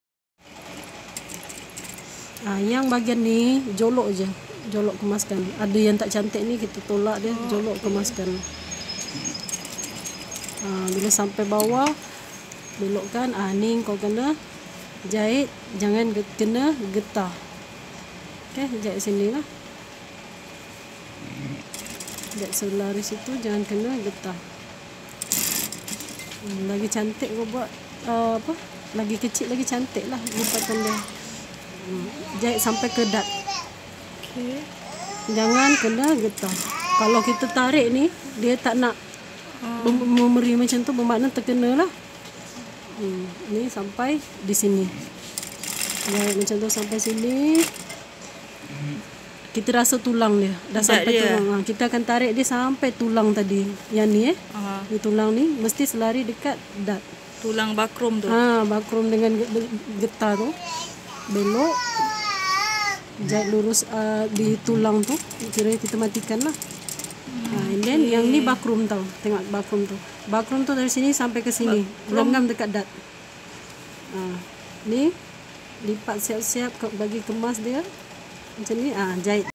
Cara Jahit Band Pinggang untuk Kain Baju Kurung Tradisional (Part 2)
Band pinggang untuk kain baju kurung tradisional adalah salah satu teknik kemasan yang boleh dijahit pada pinggang kain sama ada pattern kain tersebut adalah susun tepi untuk baju kurung, kain lipat batik untuk baju kurung kedah, kain duyung untuk kurung moden, kain kipas belakang untuk kebaya, kain kembang payung untuk peplum, kain belah tepi, kain belah tengah, kain belah belakang, dan sebagainya.